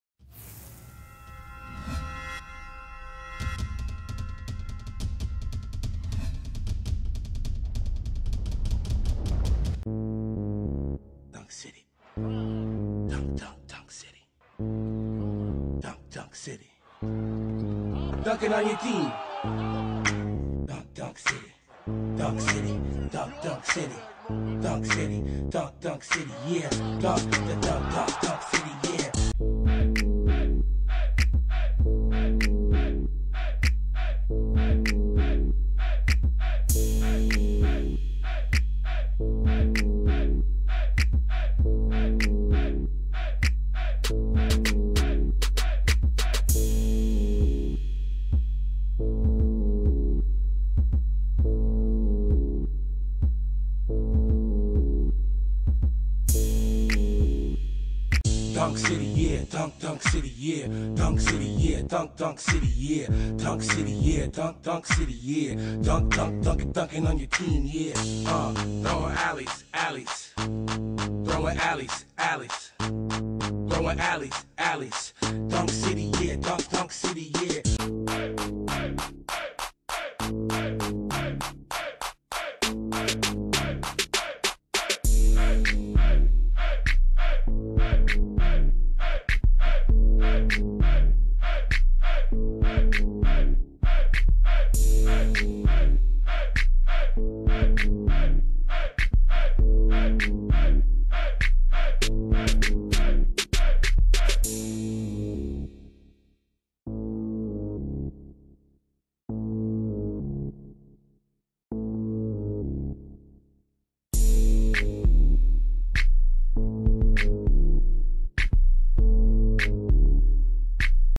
Tusculum Football Dunk Contest
Took a short time out of Spring Practice for some team competition. Dunk contest.

Tusculum Video Crew filmed the dunk contest but thanks to Tyga and FGCU!
Tusculum College does not own the Rights to these songs.